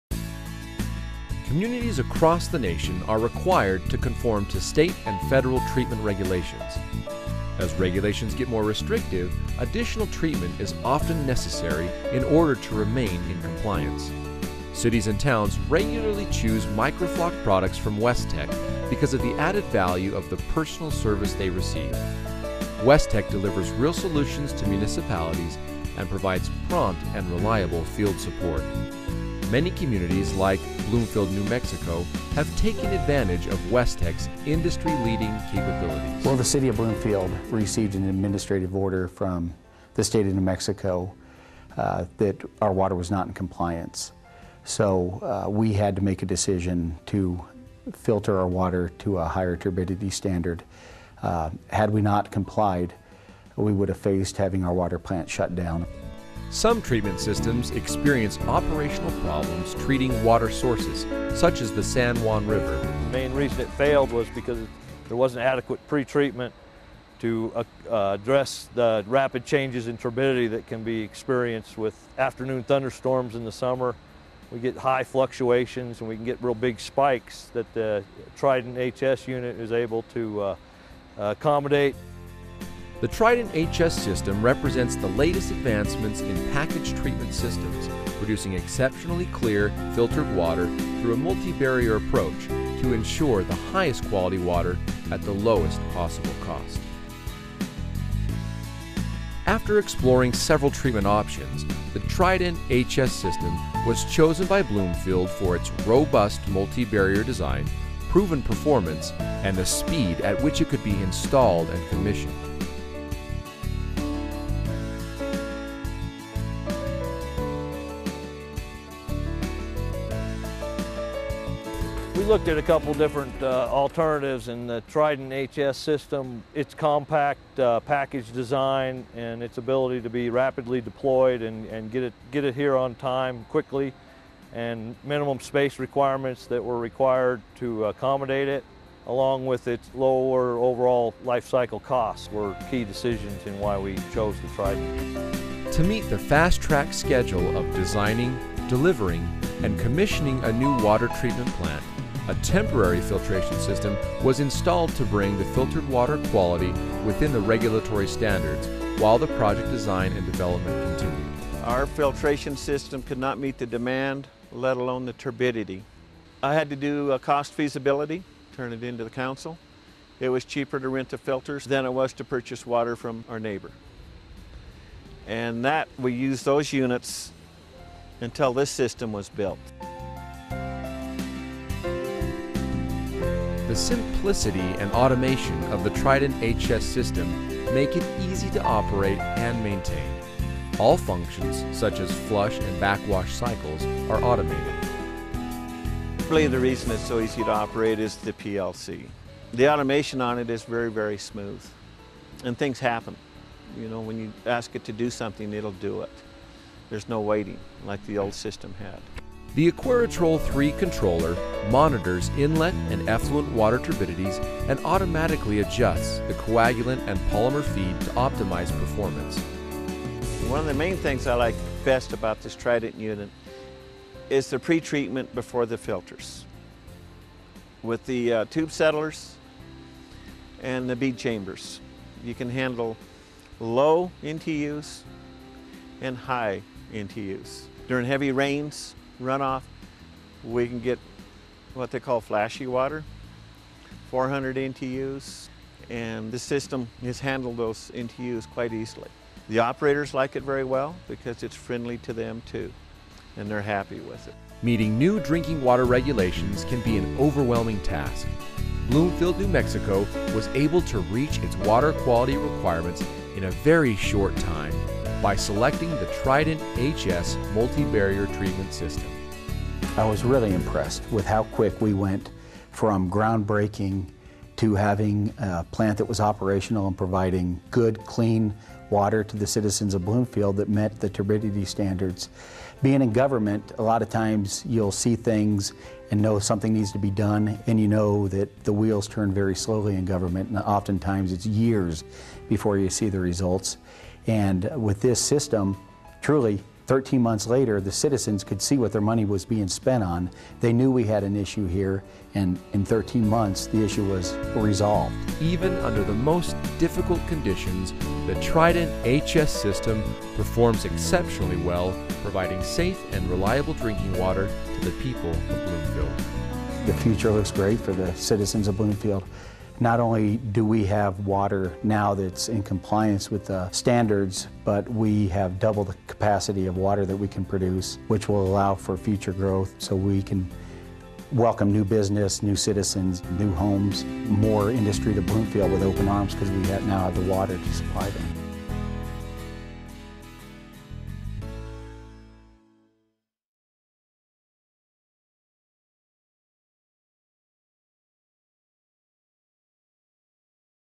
Trident HS - Bloomfield, NM
The Trident® HS package treatment system provides multi-barrier protection for difficult-to-treat surface water, groundwater, industrial process water, and tertiary wastewater. The multi-barrier design of the Trident HS, which consists of packaged high-rate settling, adsorption clarification, mixed media filtration and optional UV disinfection, is the latest improvement of the original Trident® system.